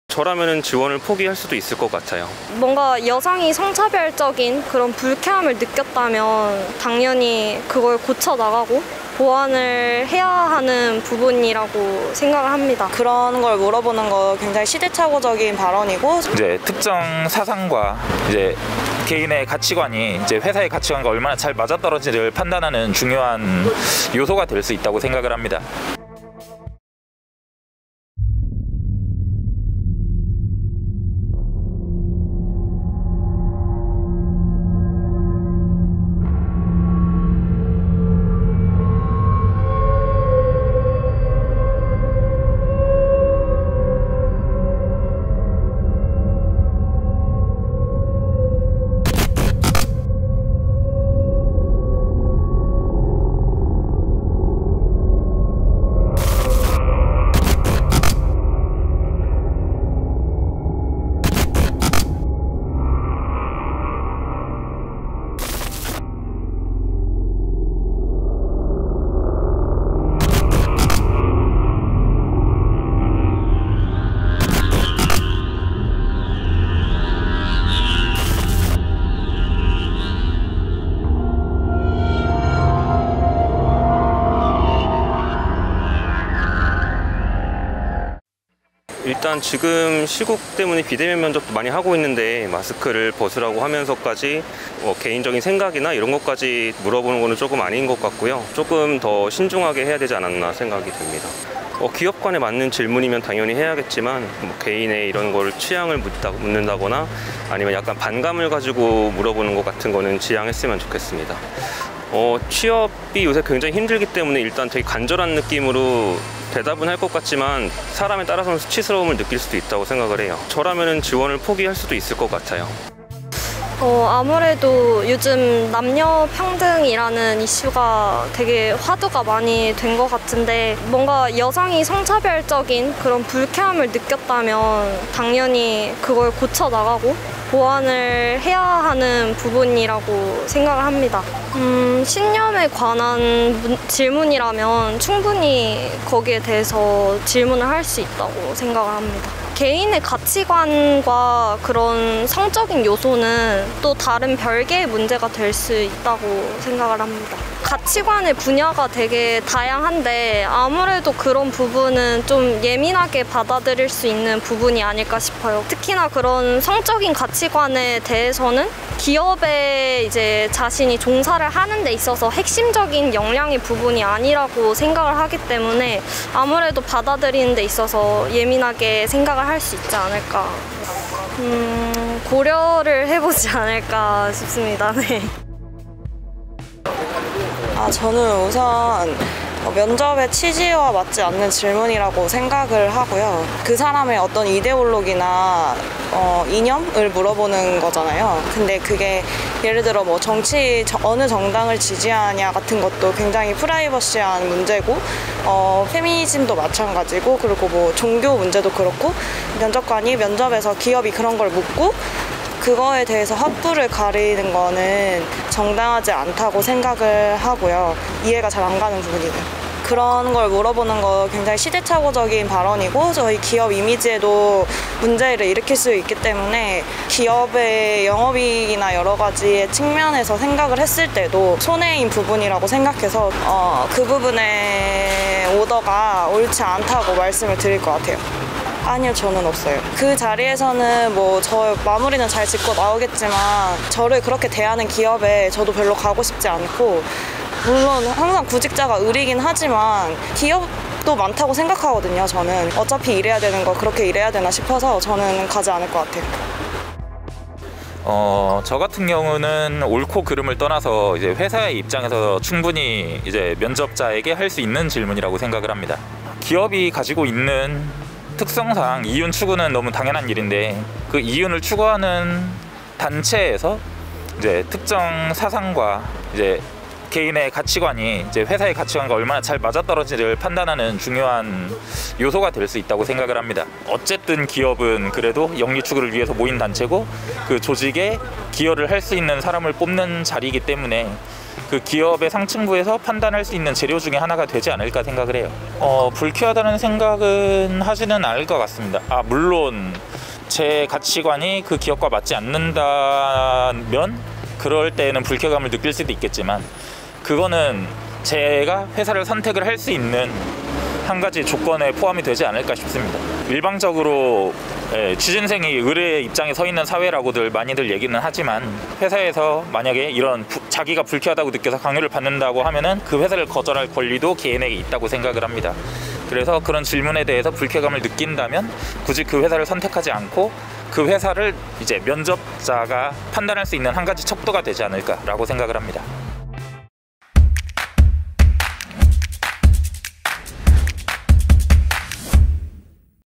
[현장영상] “시대 착오적” vs.“중요한 요소” 면접서 페미니즘 질문 면접관…시민 반응은
“여성이 성차별적인 불쾌함을 느꼈다면 당연히 그것을 고쳐 나가야죠.”
“(가치관 질문은) 시대착오적인 발언입니다.”
“(가치관 질문은) 회사와 개인이 얼마나 잘 맞는지 확인할 수 있는 중요한 요소입니다.”

한 회사가 마케팅 직원 채용 면접 자리에서
직무내용과 상관없는 가치관 관련 질문을 던졌습니다.

KBS와 SBS 등 보도에 따르면, 이 기업은 제보자에게
‘페미니즘에 대해 어떻게 생각하는가’ ‘유리천장은 있다고 생각하는가’ 등
특정 가치관과 관련한 질문을 여러 차례 했습니다.
제보자는 직무 직성과 무관한 질문을 연이어 던졌다는 입장입니다.

사측은 이런 질문을 한 사실을 인정하면서도, 페미니즘 관련 질문은 성별에 상관없이 모든 지원자에게 공통적으로 한 질문이라고 밝혔습니다.

이에 시민들은 ‘사측이 그런 질문을 할 수 있다’는 입장과
‘업무 내용과 상관없는, 개인 가치관 관련 질문은 과하다’는 입장으로 나뉘었습니다.

15일 청계광장을 찾아 시민 의견을 들어 보았습니다.